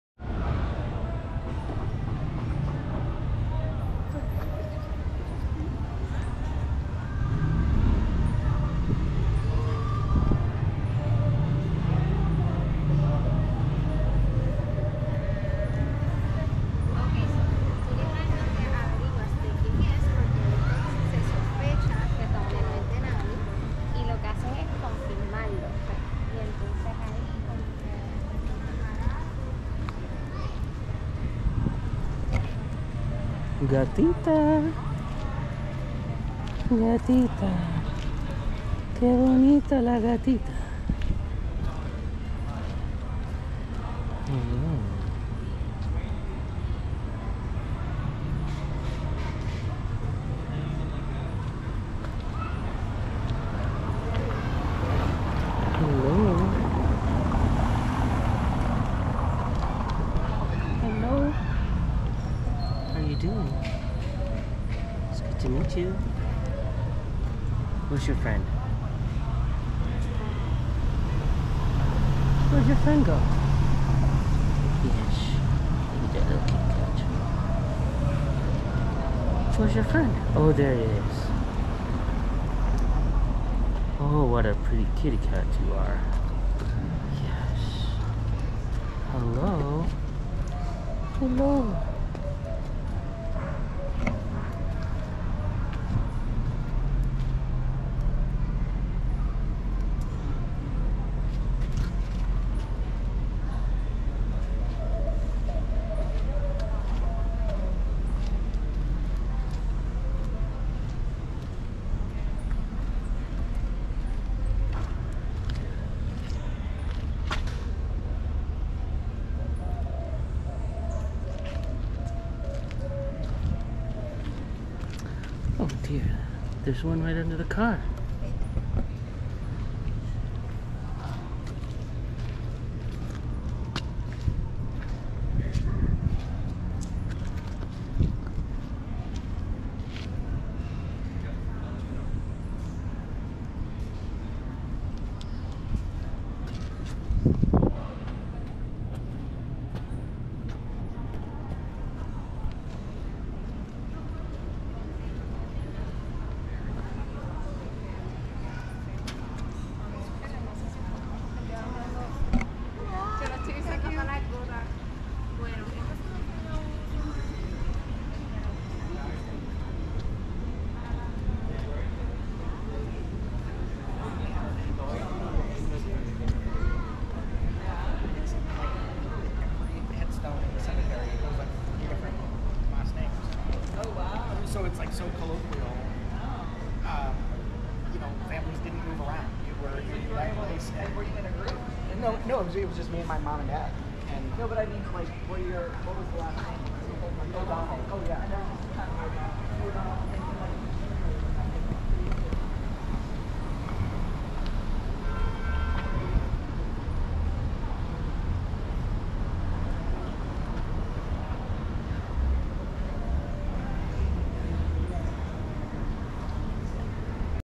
Visiting the cats of Old San Juan around Plaza de la Catedral
Visiting the cats of Old San Juan around Plaza de la Catedral on Monday, April 15, 2024.